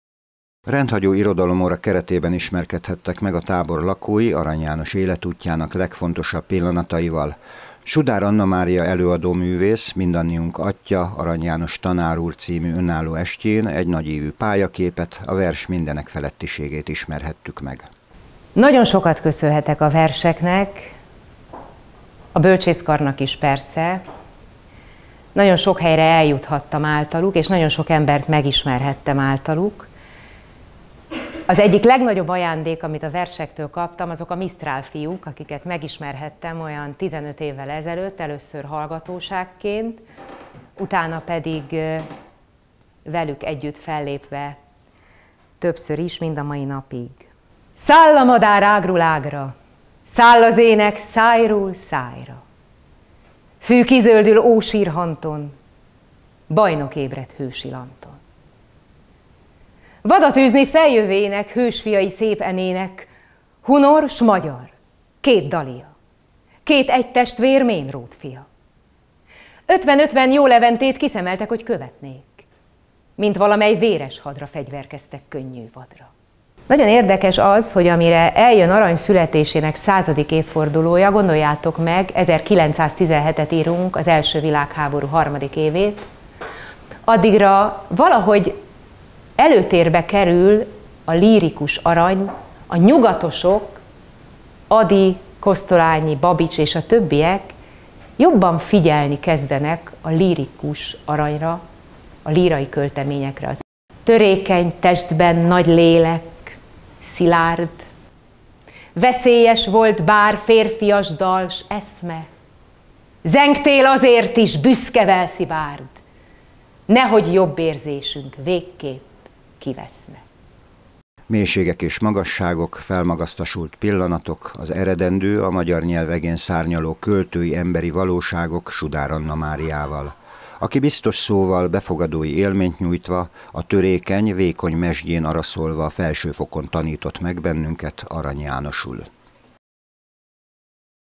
Arany János Est - Paletta
Sudár Annamária az Arany János emlékév keretében egy önálló esttel idézte meg a költőóriás szellemét Csíkcsomortánban. A műsorban Az előadó Arany János életútjának legfontosabb állomásait gyűjtötte csokorba.